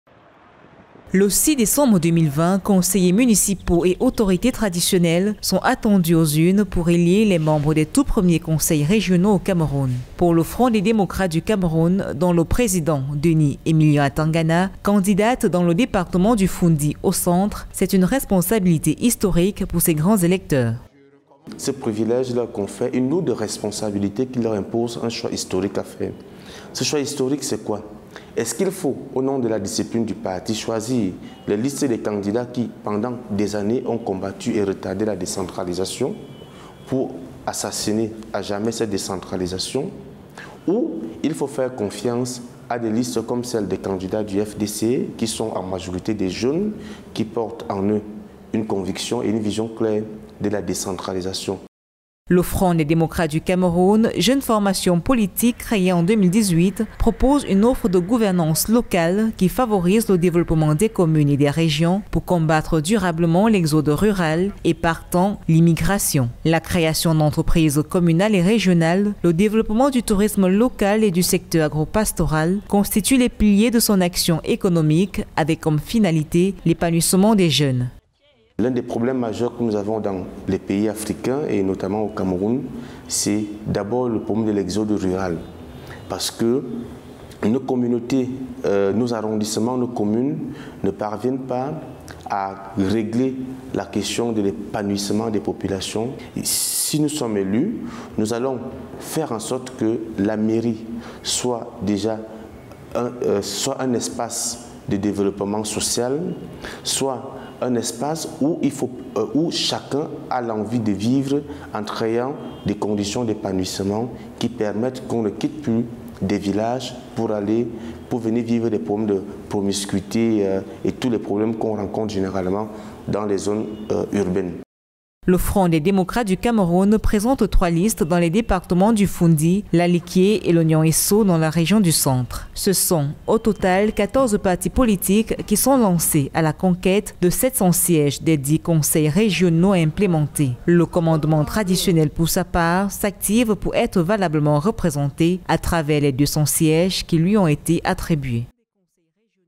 Cameroun/Régionales : Le FDC pour une gouvernance locale au service de la jeunesse
Politique, Une gouvernance locale au service de l’épanouissement des jeunes, c’est ce que propose le Front des Démocrates du Cameroun qui a présenté trois listes dans les prochaines élections régionales au Cameroun.